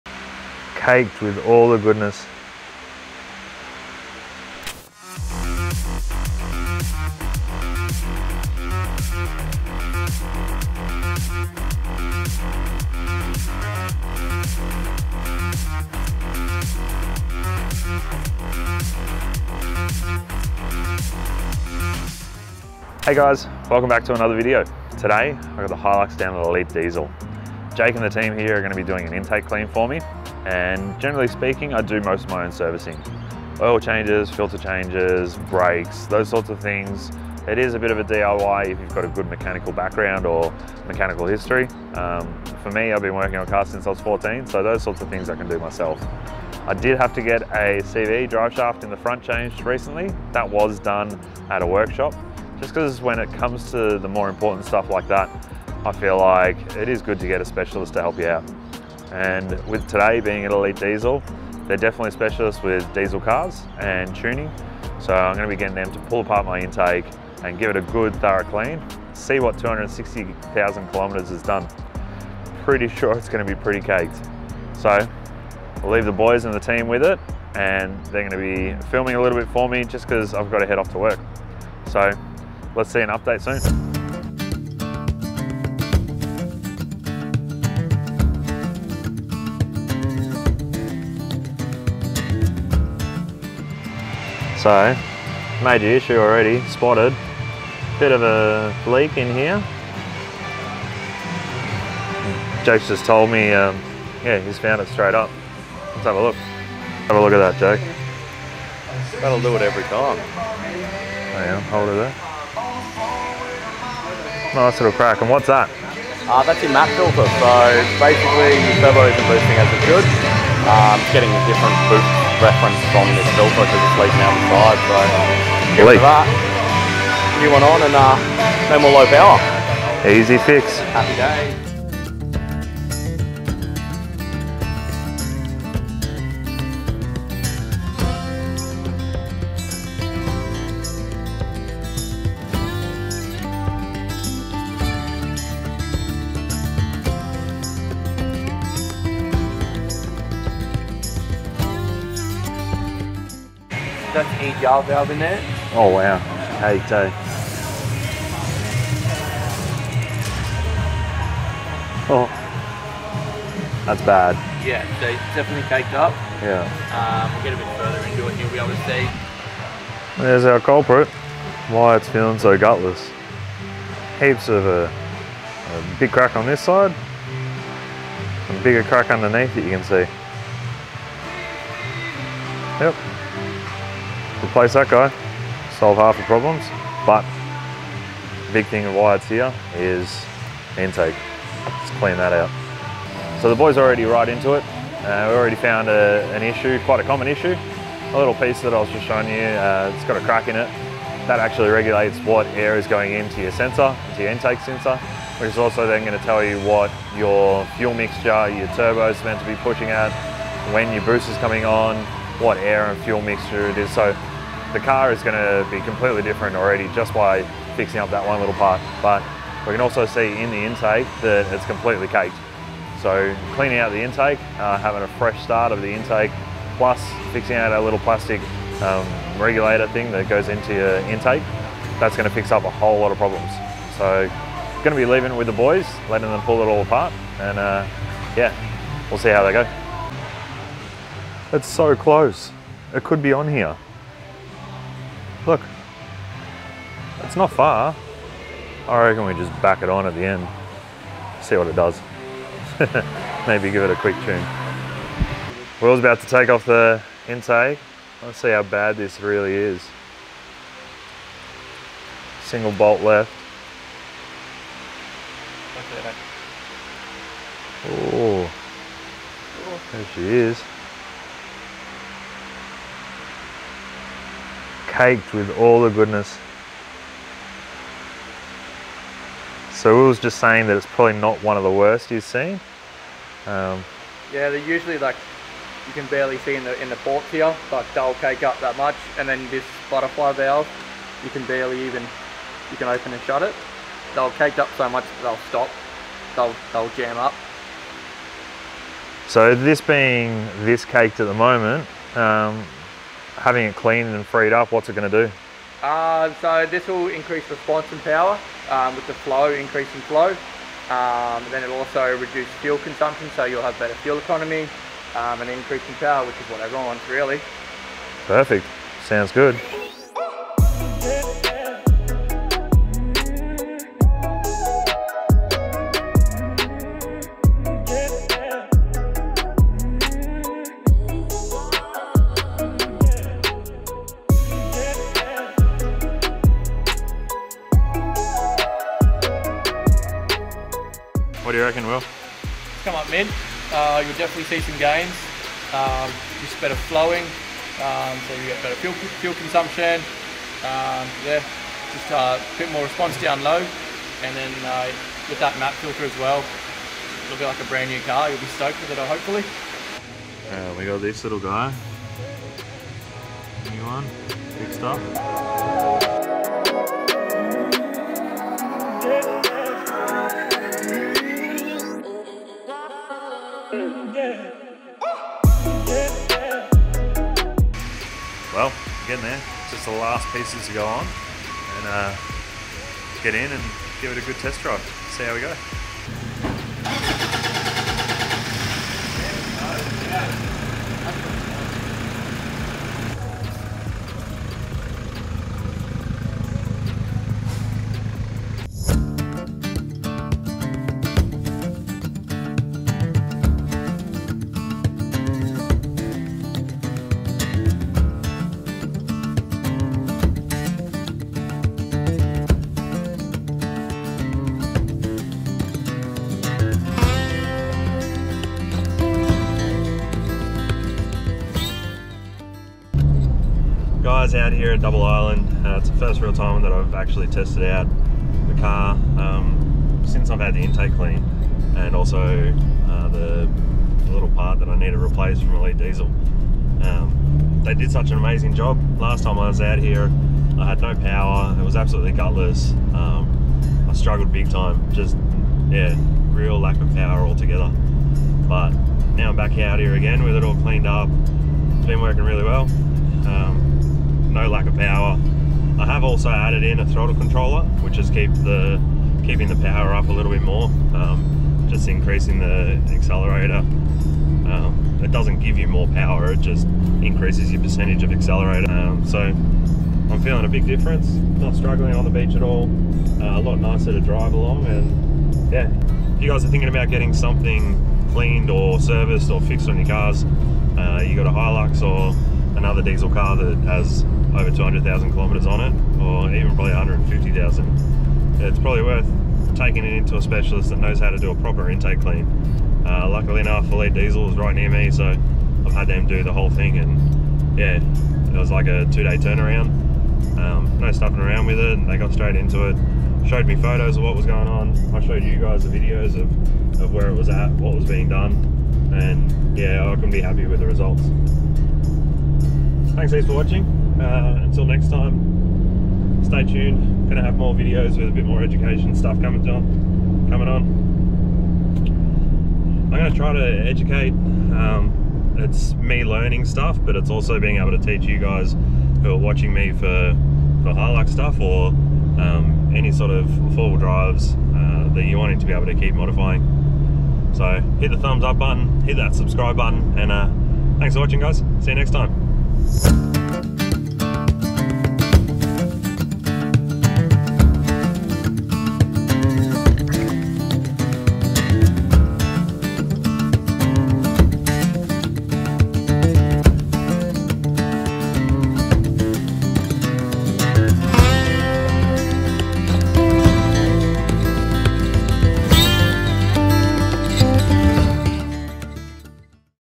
Toyota Hilux N70 D4D 3.0L Intake Clean - 4x4 offroad MediaHilux
In this video I go visit Jake and the team at Elite Diesel to get my N70 Hilux intake cleaned. It makes a massive difference and can highly recommend it.